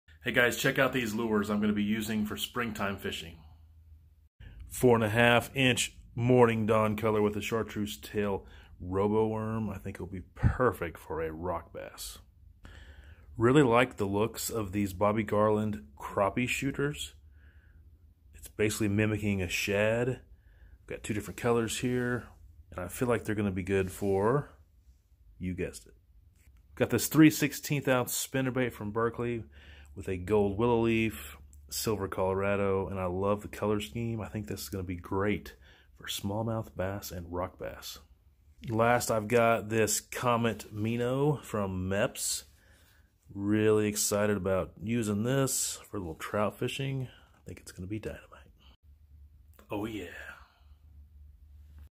Fishing lures I’ll be using for spring!
Showing off my new lures for spring time fishing!
The Comet Mino from Mepps, the Roboworm, Bobby Garland Crappie Shooter, and the Berkley 3/16 oz. Spinner bait.